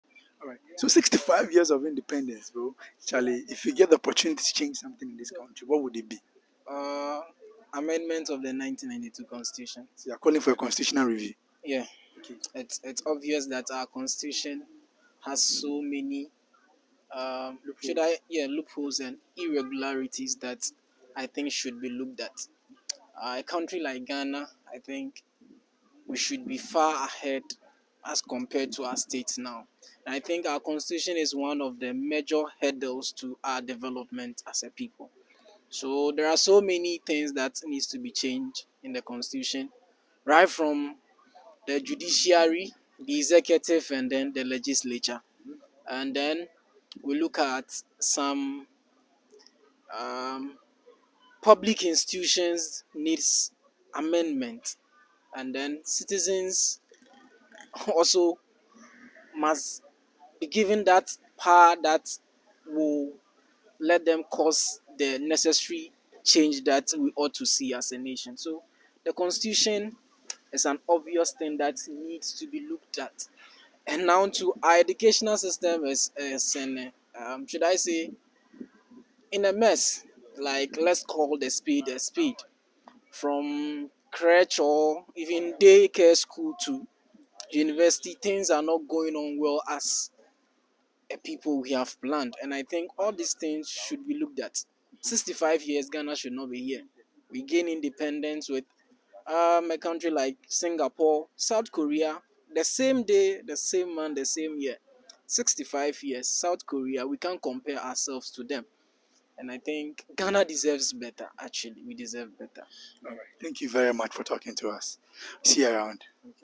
65 Years of Independence:If you had the power to change anything in Ghana,what would it be#GhanaAt65
As part of Ghana month celebration we decided to interact with students on matters about our dear motherland. We asked students of the university ghana legon to tell us what they would love to see change in Ghana after 65 years of independence.

Alor Justice is the current majority leader of university ghana Parliament House and he’s calling for a constitutional review and reforms in our educational sector.